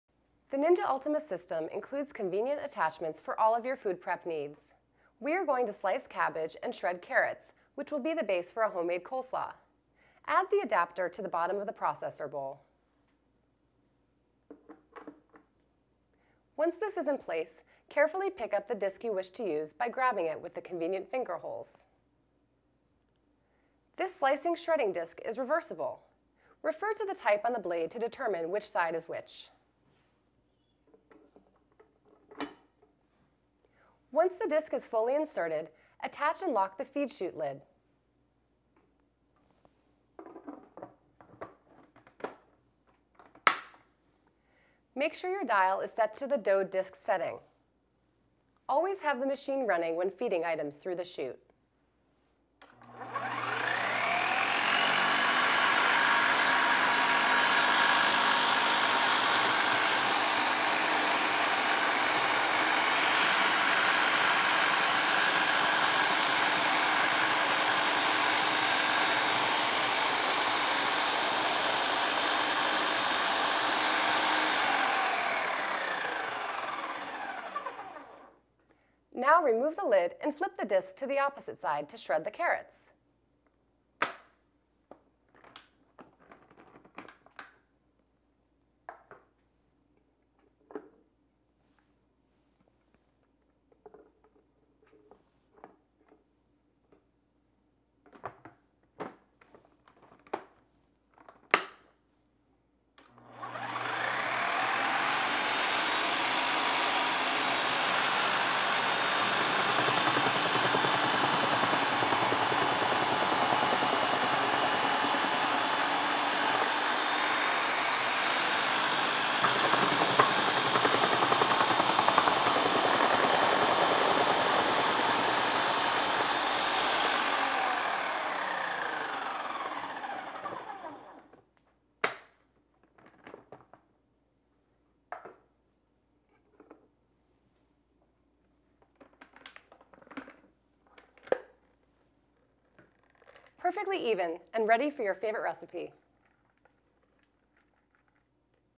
The Ninja Ultima Blender: Coleslaw Recipe
With the Ninja Ultima Blender, making your own coleslaw is easy.  Follow our simple recipe.

For even more great recipes that you can make with your Ninja Ultima Blender,